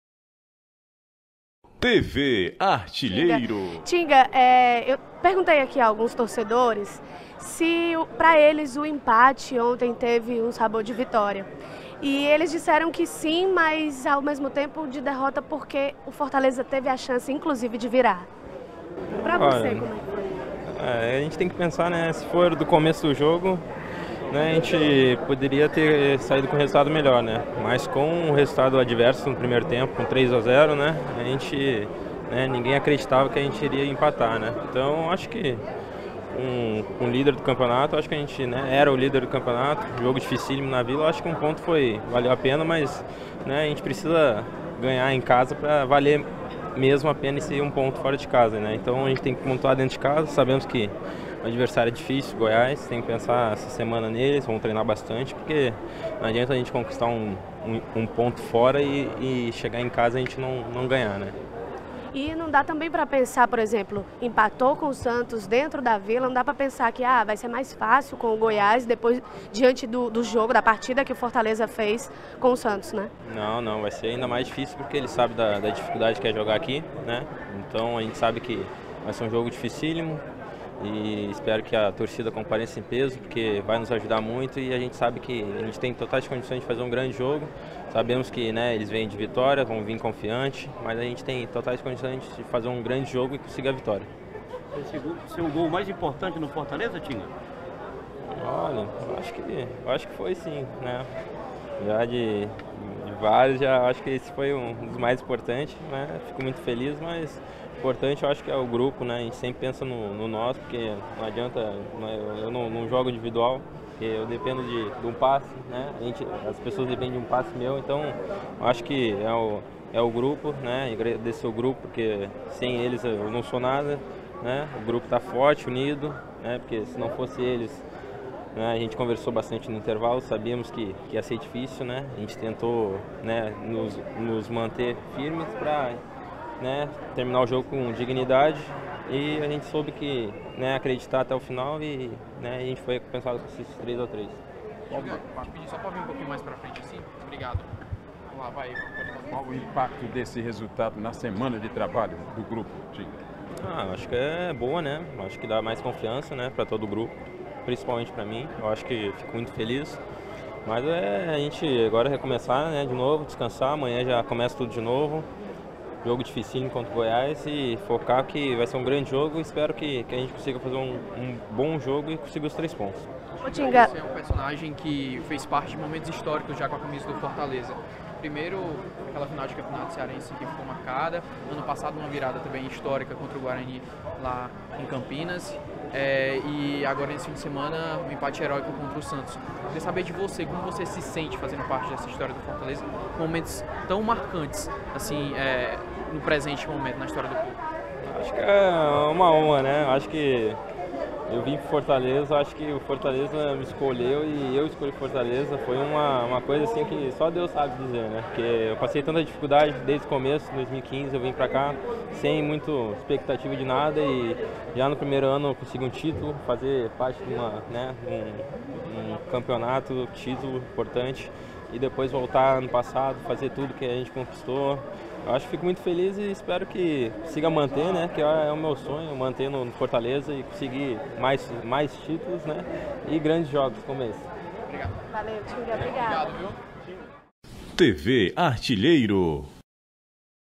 [Série A '19] Coletiva Tinga | Desembarque | Pós-jogo Santos 3 X 3 Fortaleza | TV ARTILHEIRO
[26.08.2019] Entrevista Coletiva Lateral Tinga |
Pós-jogo Santos Futebol Clube 3 X 3 Fortaleza Esporte Clube |
Campeonato Brasileiro | Série A | 16ª Rodada |